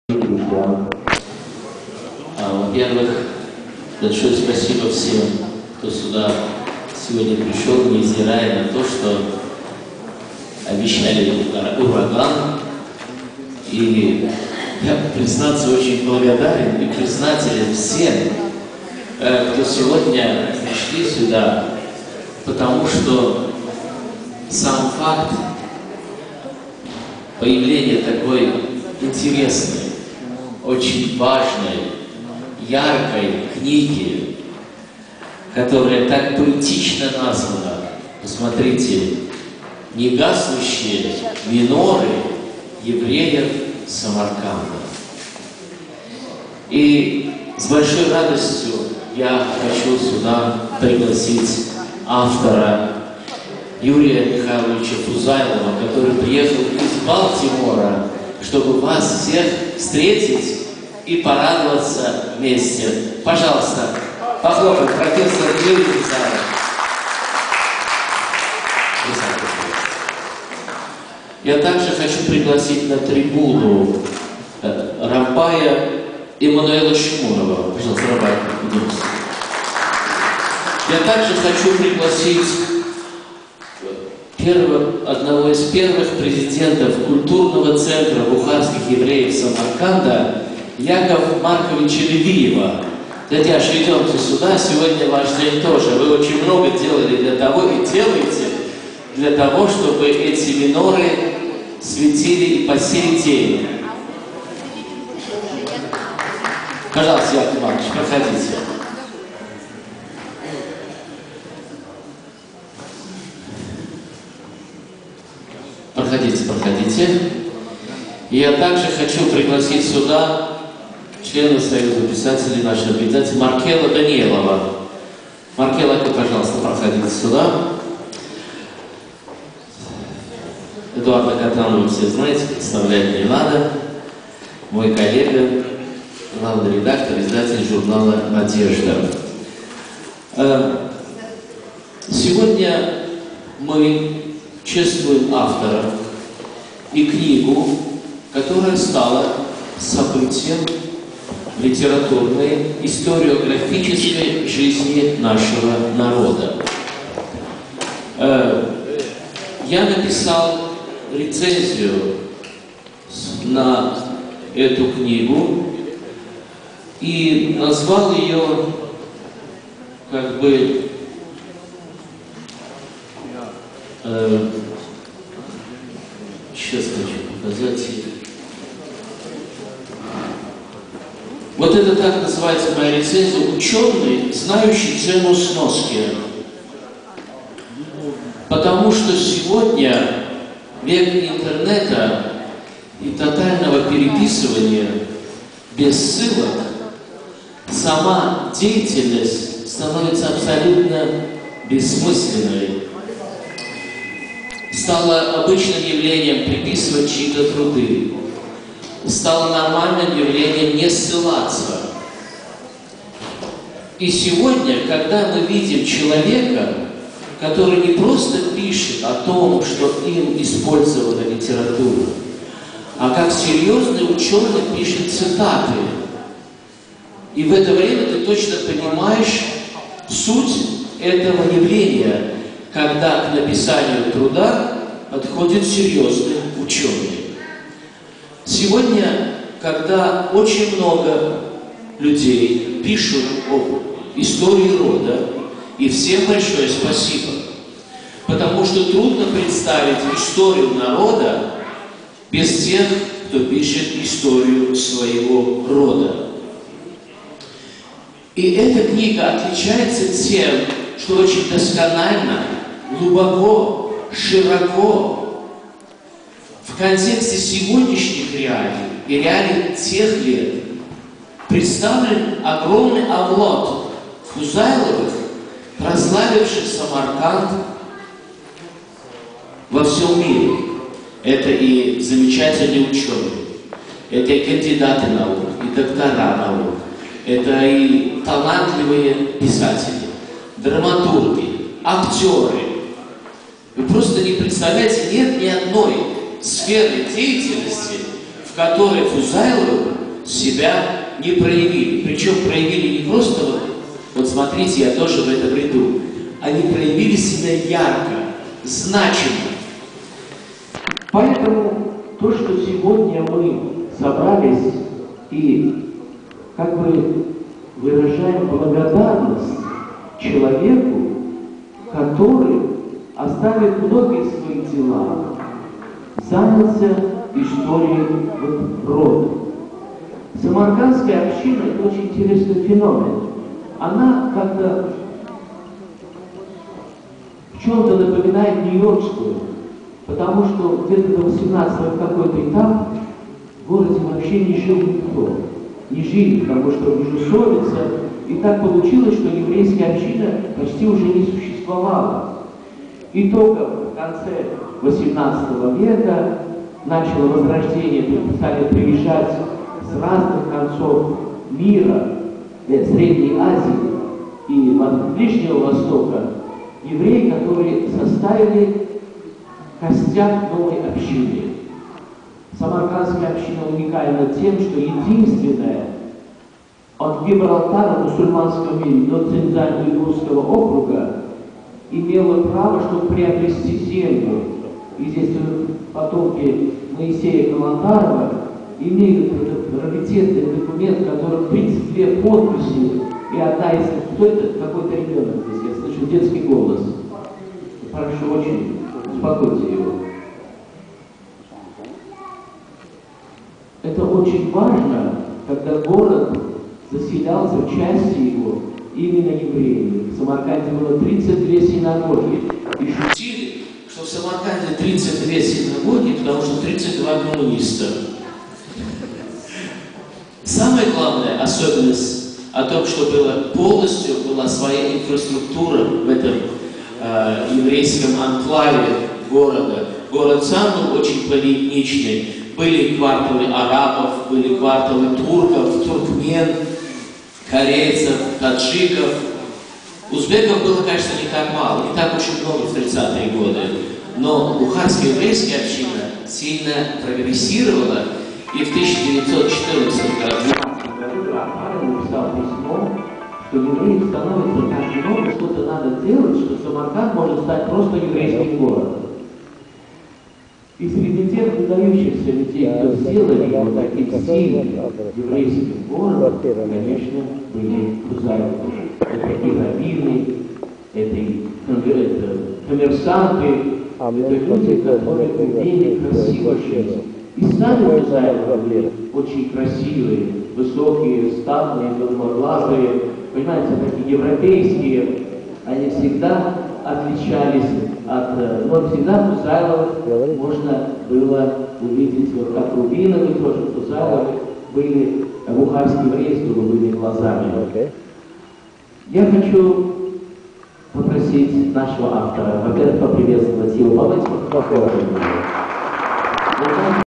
001.MPG, Misha Normatov, CHEIF EDITOR BUKHARIAN TIMES-RAFAEL NEKTALOV
PRESENTATION OF BOOK "NEGASAUSHIE MENORI BUKHARSKIE JEWISH SAMARKANDA" - OF YURIY MIKHAILOVICH FUZAYLOV FROM BALTIMORE- PROFESSOR - CANDIDATE MED. NAUK  IN BUKHARIAN CENTER SYNAGOGUE-QUEENS, NY